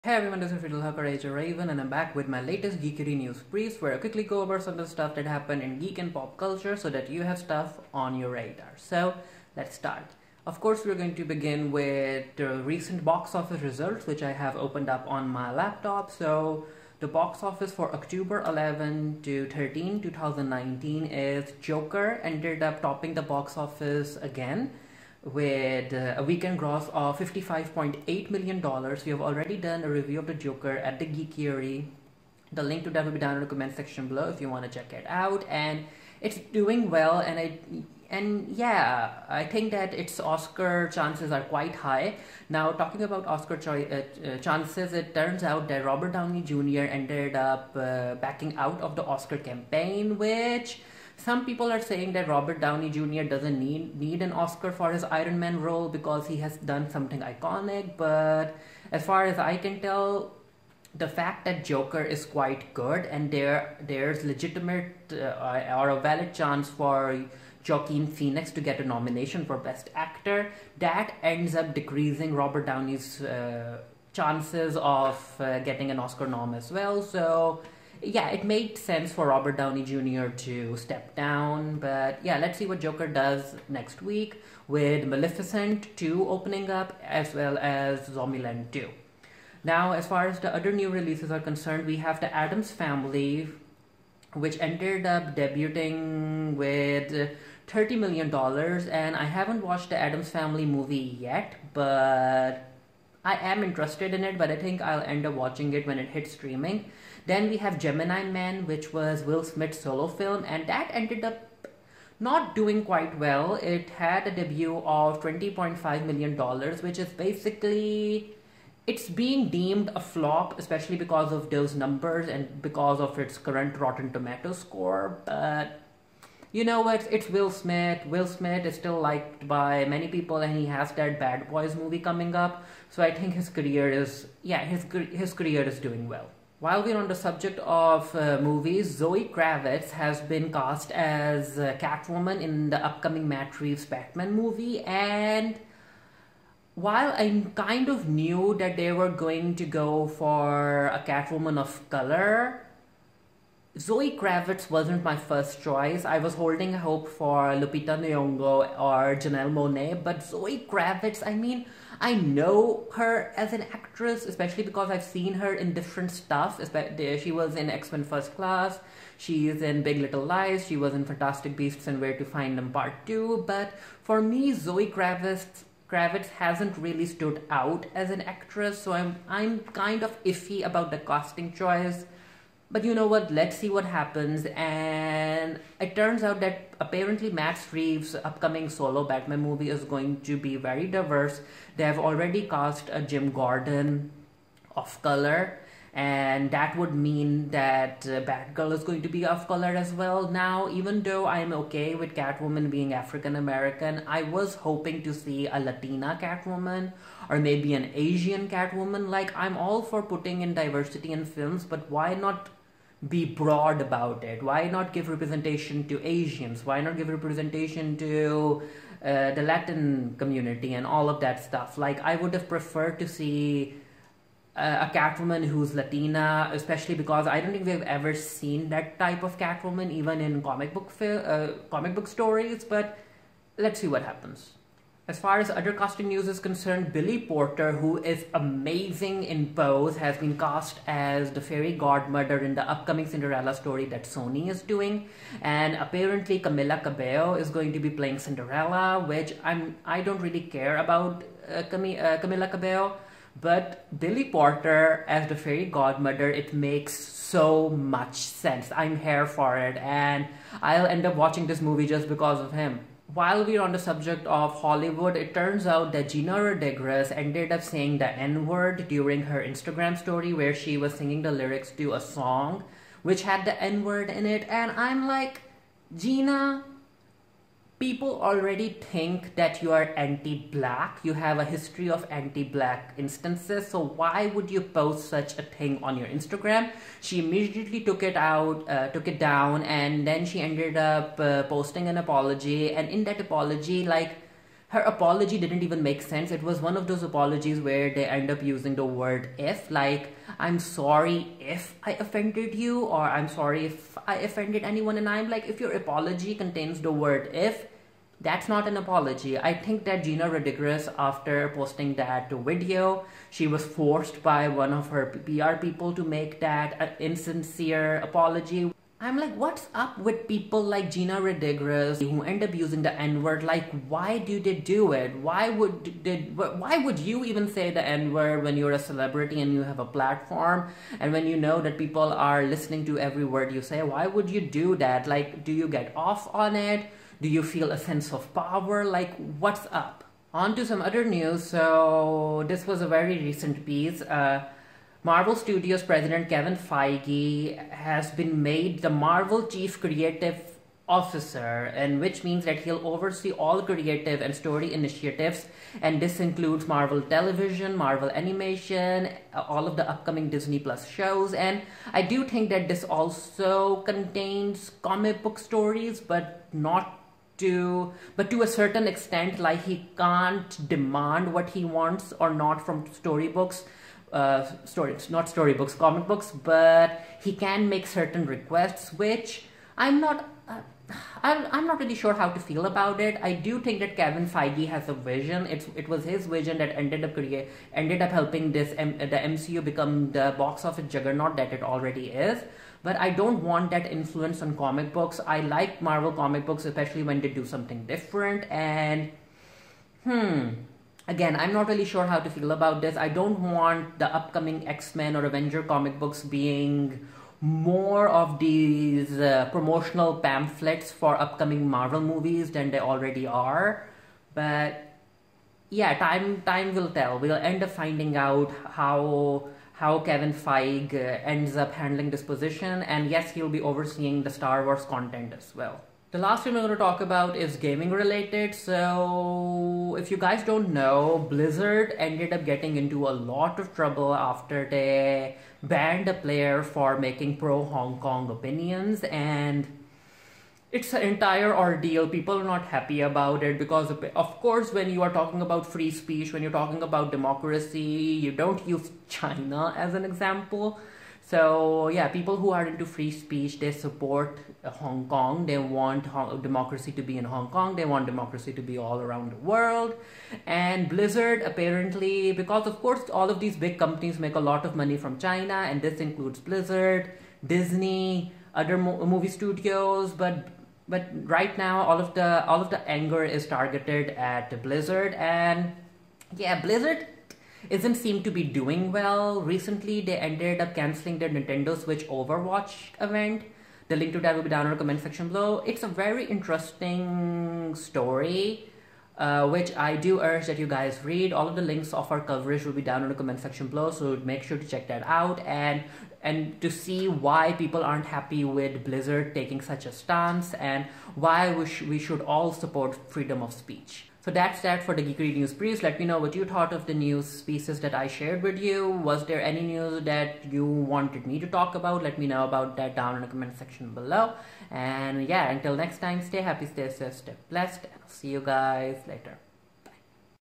Zoe Kravitz is Catwoman! Kevin Feige Controls Everything Marvel? And More!
The Geekiary News Briefs (October 15, 2019). I cover Zoe Kravitz being cast as Catwoman in the upcoming Batman film. Kevin Feige has been given the job of Marvel Chief Creative Officer. Gina Rodriguez seems to continue being anti-black? Billy Porter is the Fairy Godmother in Sony's Cinderella! And More!